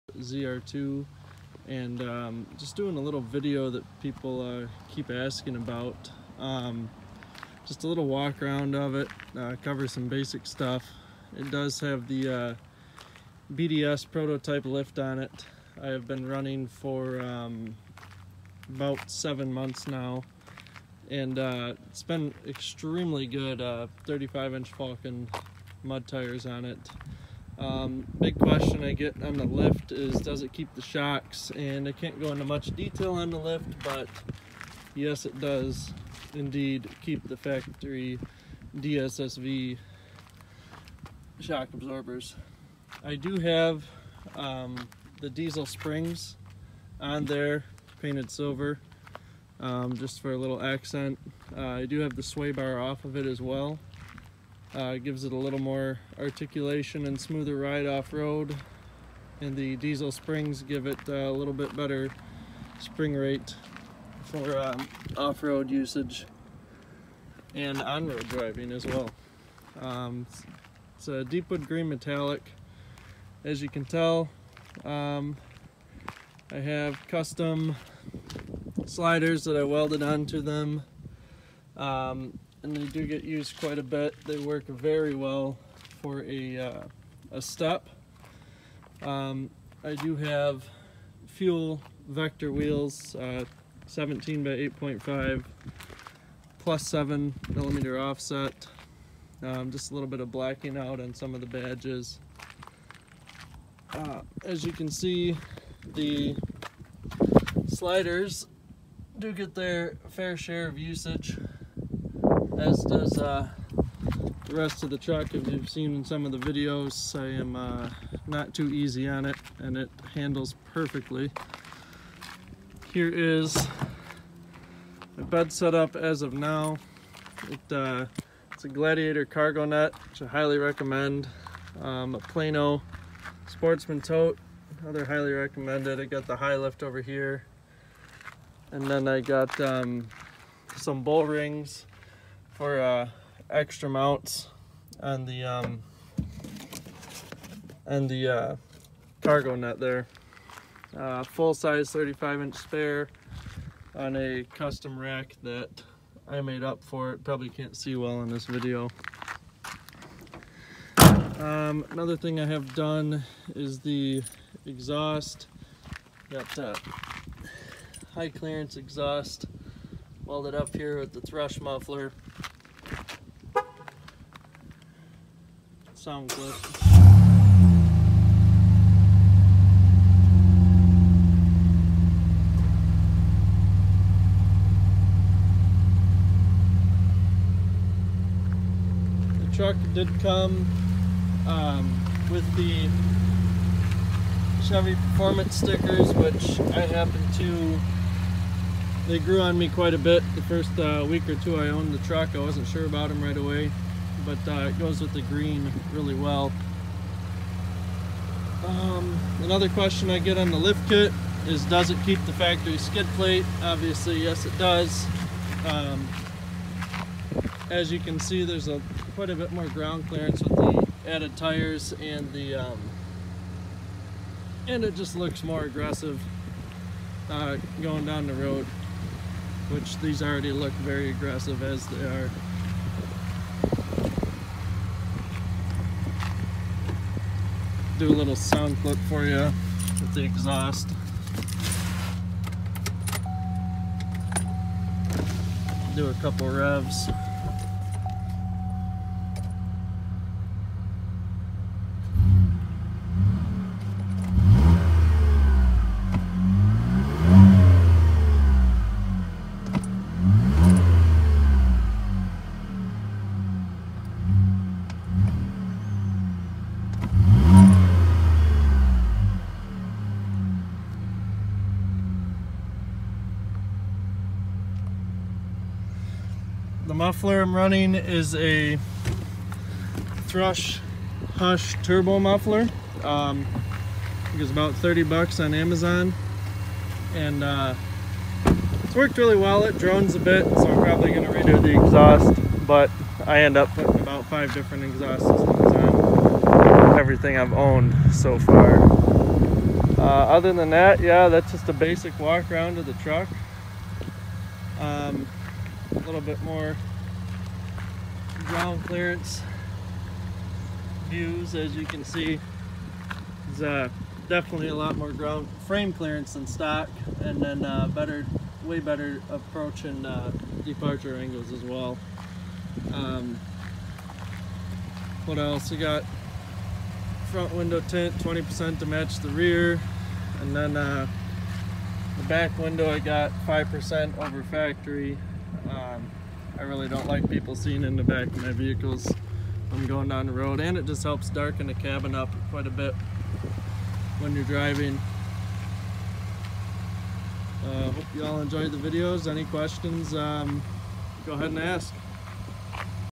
ZR2 PROTOTYPE LIFT on 35s WALK AROUND!
As requested...walk around of my ZR2 on 35s with the bds prototype lift